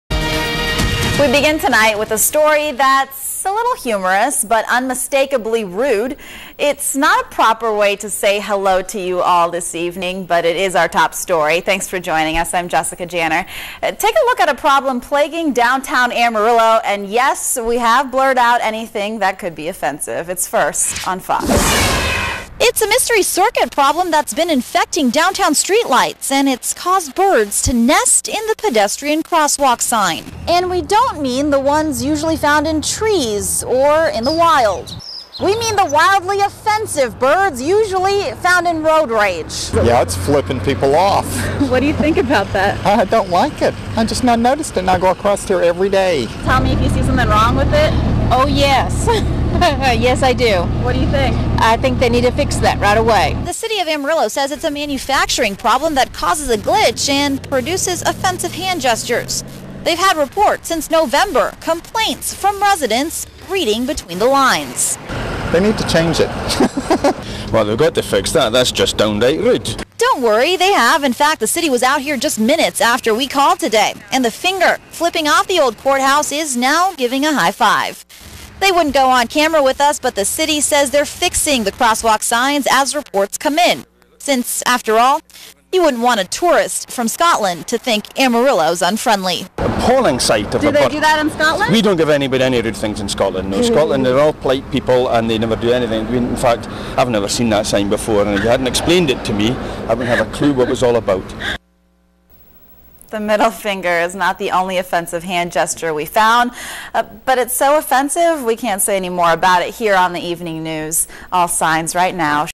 Cross Walks Give Middle Finger
It's a mystery circuit problem that's been infecting downtown street lights and giving residents the middle finger.